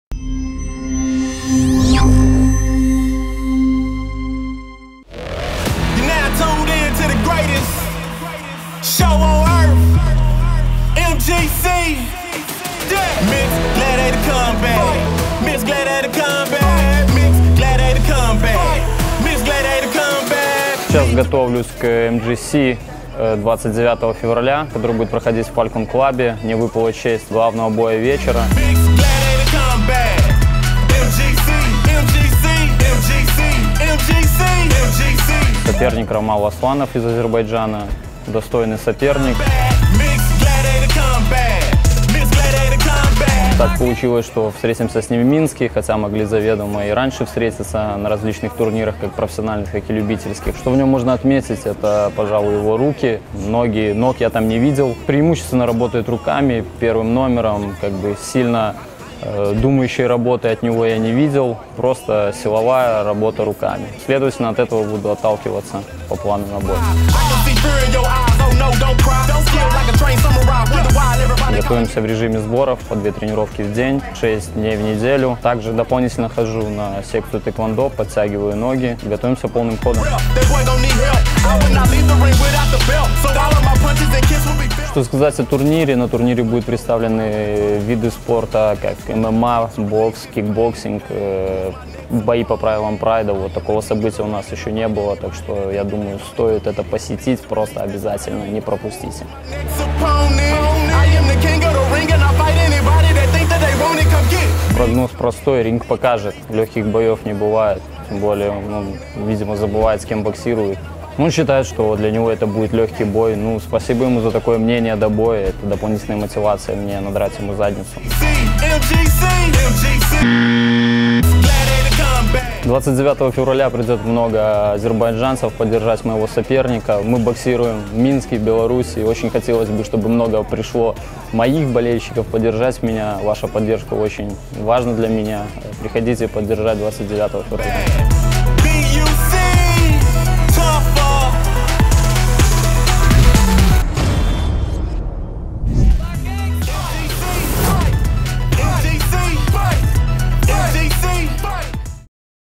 MGC: Live 2 Fight | Maxim Spodarenko Official Promo
Максим Сподаренко: Высказывания Рамала Асланова про меня – дополнительная мотивация надрать ему задницу
«Прогноз на бой с Рамалом Аслановым у меня простой: ринг все покажет. Легких боев не бывает. Тем более, что азербайджанец видимо забывает, с кем будет боксировать. Он считает, что для него это будет легкий бой. Что же, спасибо ему за такое мнение до боя. Это дополнительная мотивация надрать ему задницу», - сказал Максим Сподаренко.
Напомним, что ранее Рамал Асланов в своем интервью назвал белоруса физически слабым, а их бой легким, который с большой долей вероятности закончится нокаутом.
Торопитесь поддержать любимого бойца! Приобретая билеты в кассах или на сайте kvitki.by, назовите фамилию любимого бойца, за которого будете болеть и получите скидку в 10%.